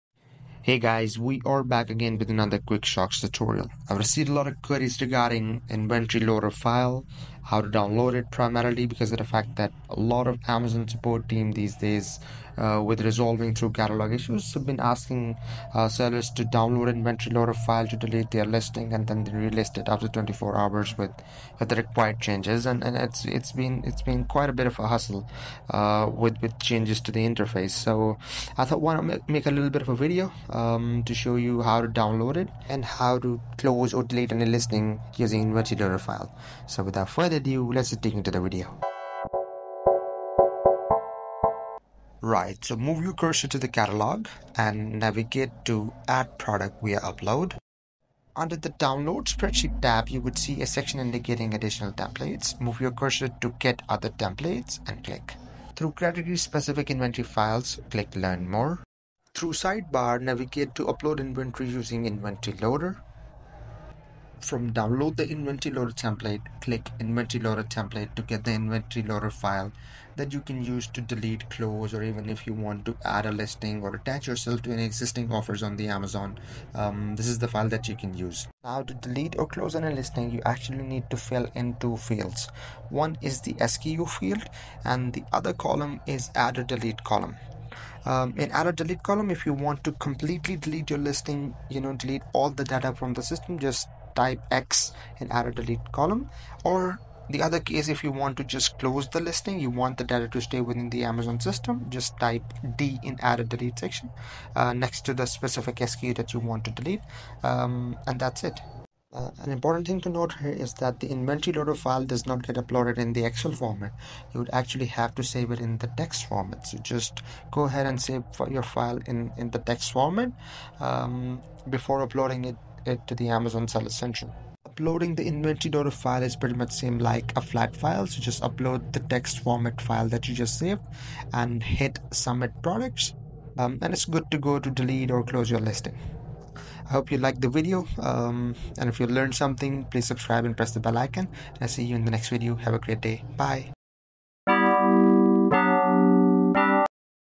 Delete or Closing Listing in Bulk Using Inventory Loader File - 2024 Updated Interface
Welcome to our comprehensive guide on navigating the latest Amazon Seller Central interface! In this video, we'll dive deep into the process of downloading, editing, and uploading the inventory loader file. Whether you're a seasoned seller or just starting out, mastering this process is crucial for efficiently managing your inventory on Amazon.

We'll walk you through each step, from accessing the inventory loader file to making necessary edits and finally uploading it to Seller Central. By the end of this tutorial, you'll be equipped with the knowledge to streamline your inventory management process and ensure smooth updates to your Amazon listings.